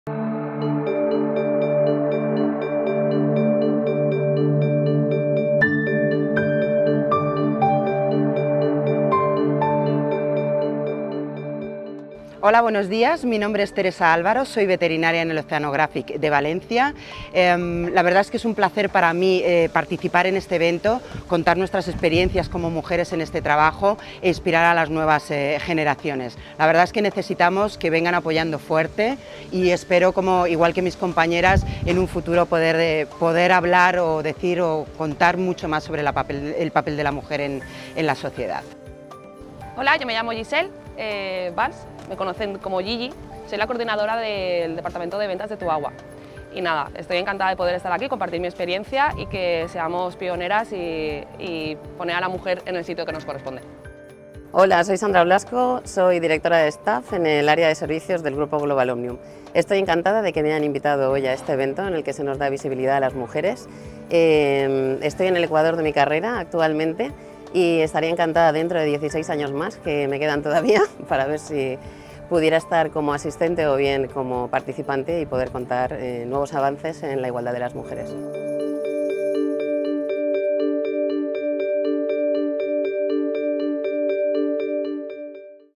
Global Omnium incrementa la presencia de mujeres en su plantilla un 38% en 7 años
Actualmente, 948 mujeres integran la compañía líder en la gestión del ciclo integral del agua. La presencia de mujeres en el grupo presidido por Eugenio Calabuig roza el 30% de la plantilla total.